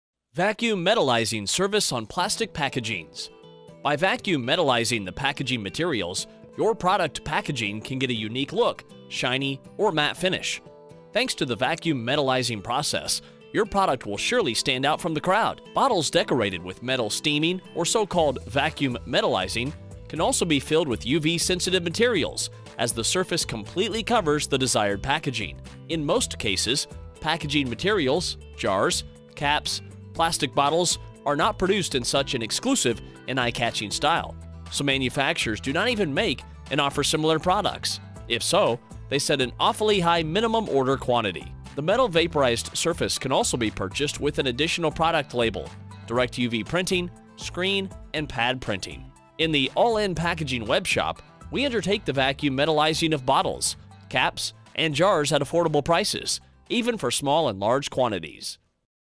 Vacuum Metallizing Service on Plastic Packaging
By vacuum metalizing the packaging materials, we get a unique look and a bright and shiny end result that helps your products stand out.